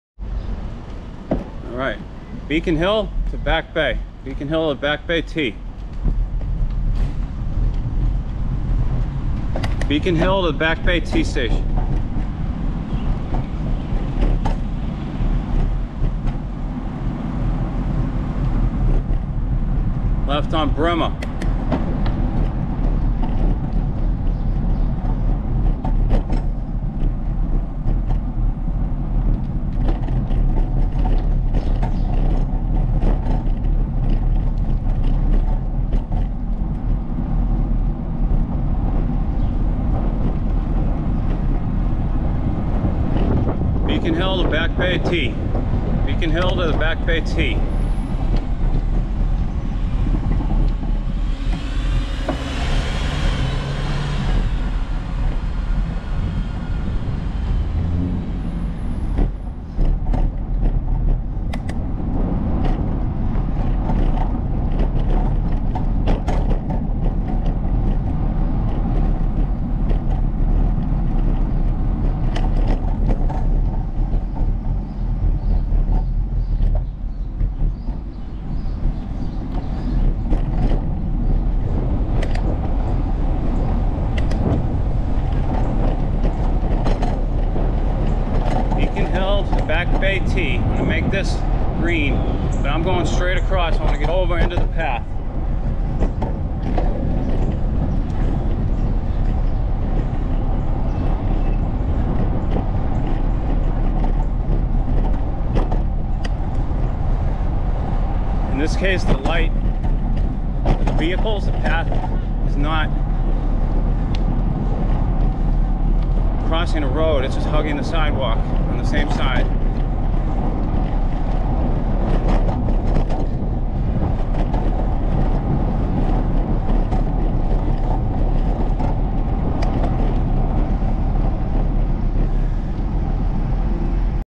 Boston Bike Route - Beacon Hill to Back Bay Station pt 1
part 1 from bouncin off the charles through a nifty way out to get all the way to the Back Bay Station trainstation . . . . . . careful on this complete route, more advanced not for kids looking for a friendly stroll